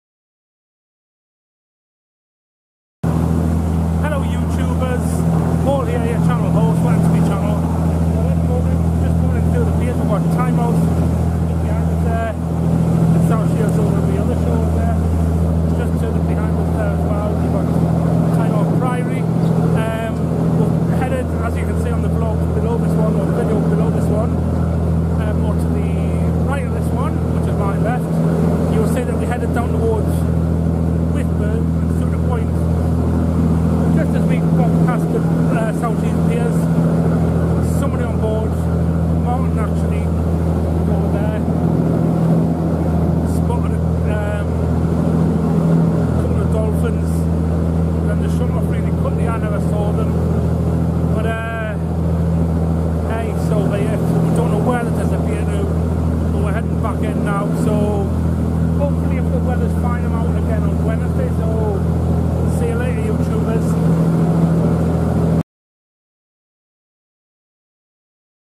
Coming In Through The Piers After Being Out On The JFK Two And Me Not Seeing The Dolphins But Other
Coming in through the piers after being out on the JFK Two and me not seeing the dolphins but other people seeing them.